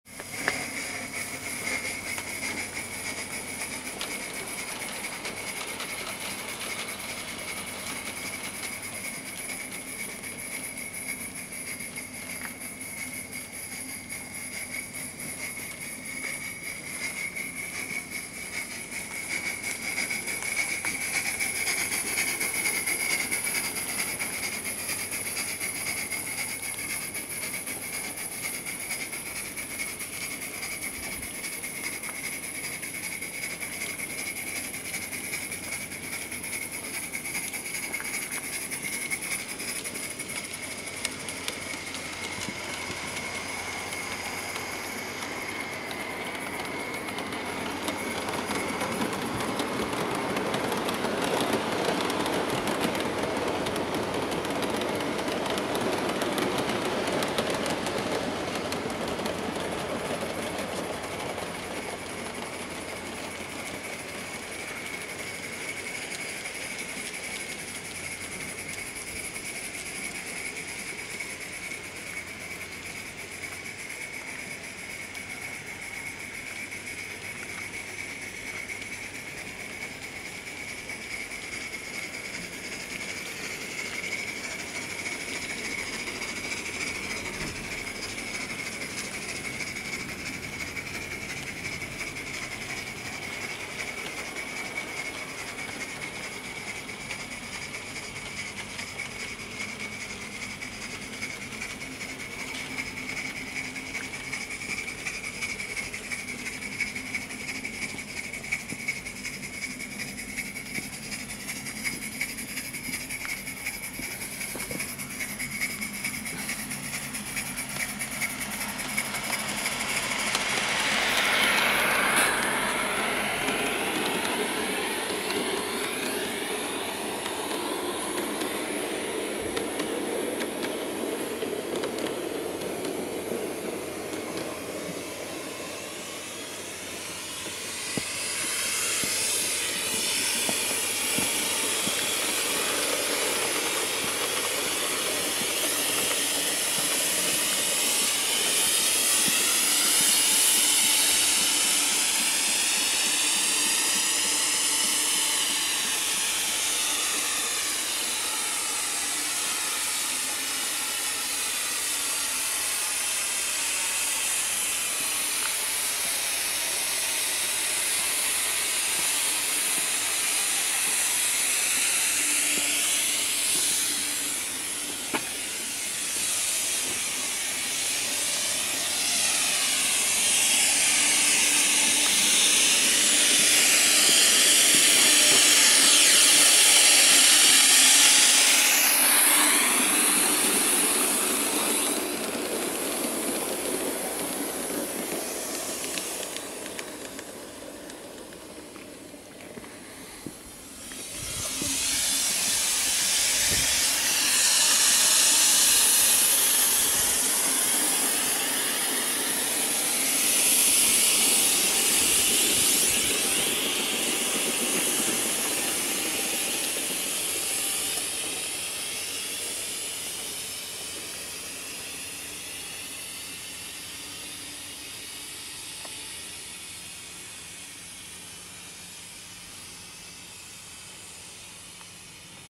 Accucraft K28 Coal fired with San Juan Cars
One rounde with the San Juan Express and Otto Mears at the Rio Grande LIVE STEAM LINE.